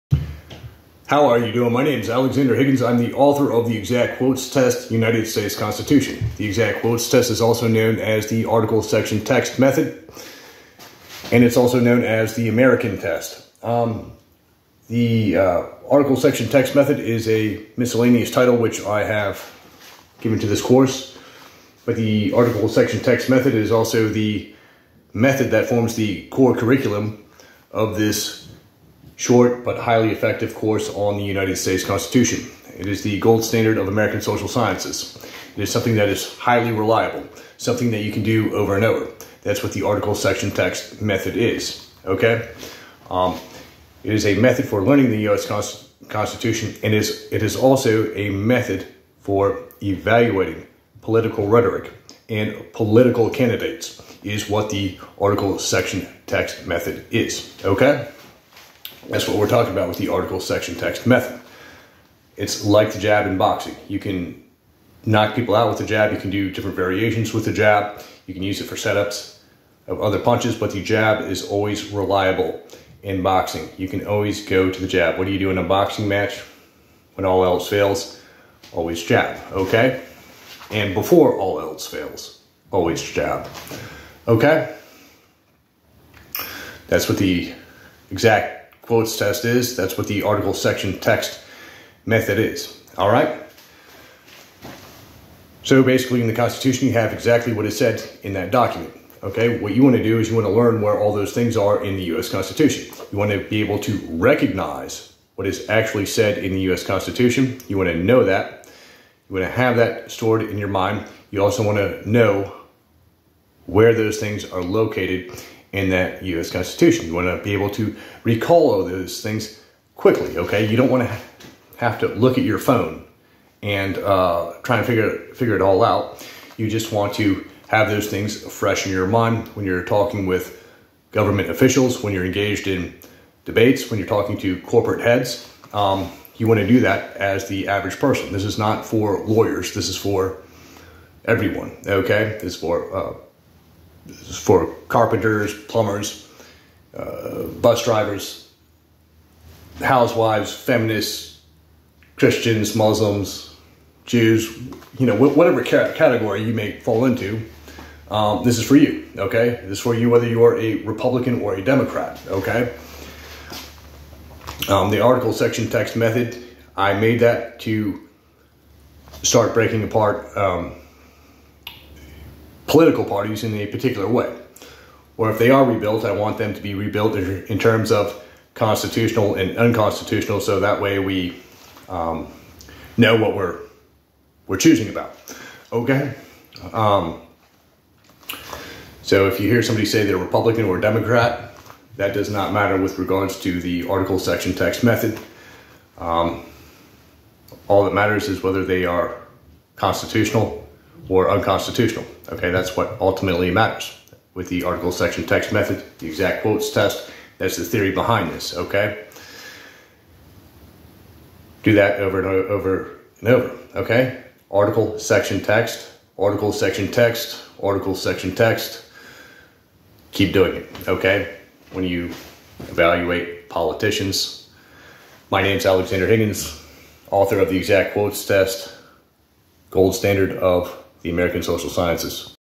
The Article Section Text Method for the Constitution
The Article-Section-Text Method:

The video describes a simple and effective method for learning the Constitution and deciding the constitutionality of actions and statements.

Rather than classifying political matters as Republican or Democratic, define them as constitutional or unconstitutional using the Article-Section-Text Method.

-- Alexander Higgins

The Exact Quotes Test United States Constitution

The American Test United States Constitution